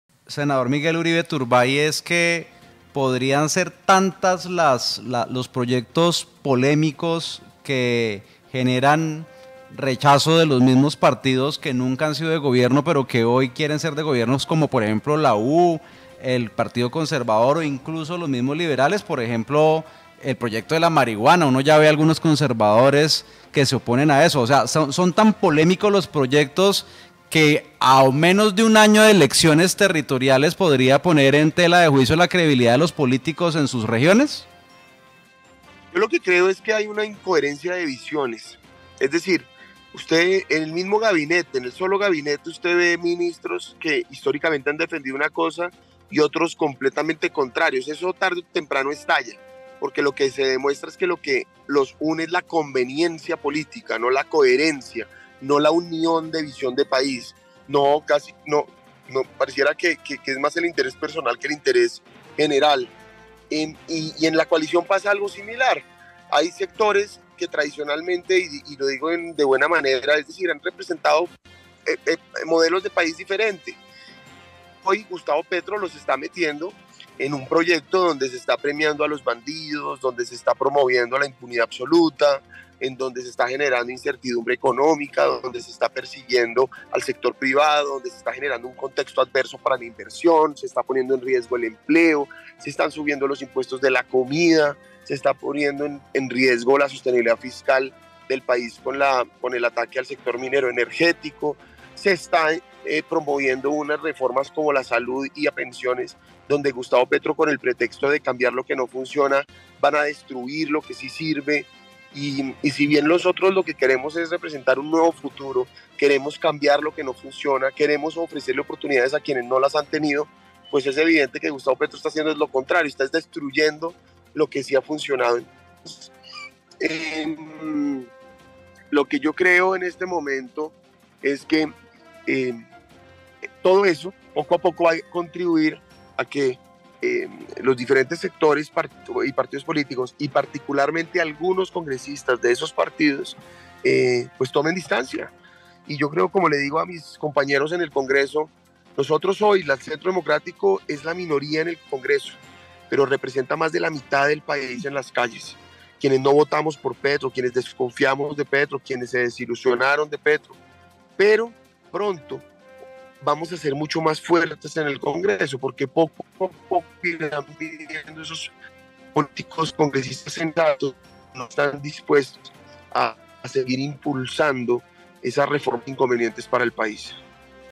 “Lo que los une es la conveniencia política, no la coherencia”: Miguel Uribe sobre coalición de Gob
“Lo que los une es la conveniencia política, no la coherencia”: Miguel Uribe sobre coalición de Gobierno